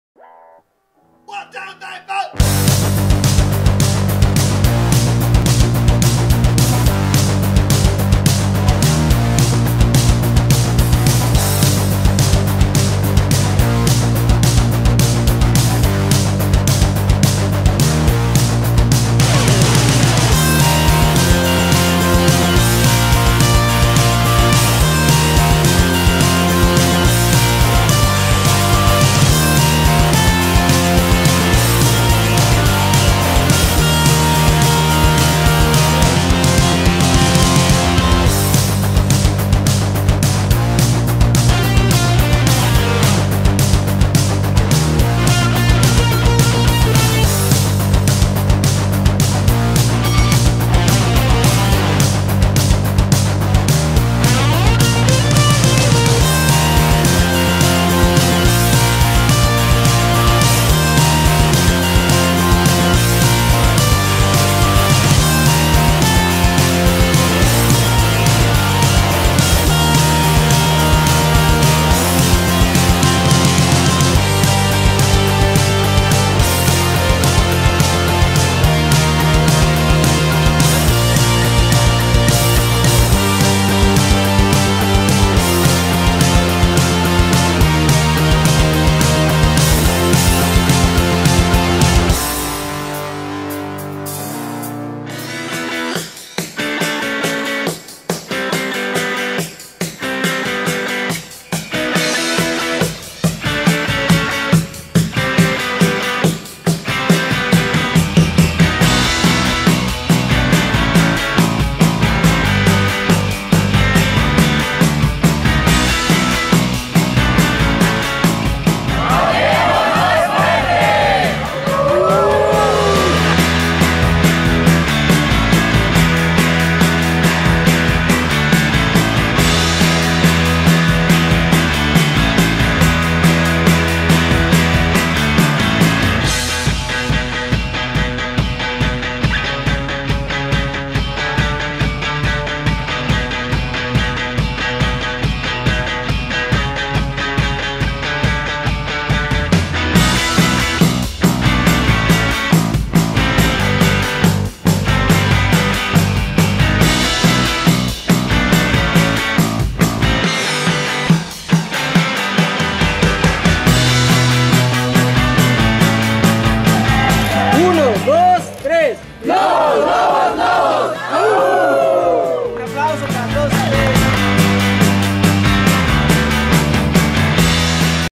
HIGHLIGHTS || CONVIVENCIA LOBOABONADOS 19
Revive aquí los mejores momentos de nuestra convivencia con Loboabonados de este Clausura 2019.

Nuevamente le damos las gracias a nuestros patrocinadores oficiales Energain y Gatorade por acompañar a nuestros abonados en esta increíble experiencia.

También gracias a la jugadoras de Lobos BUAP Femenil, por su participación en este evento.

Próximamente, espera información sobre nuestro abono del próximo torneo.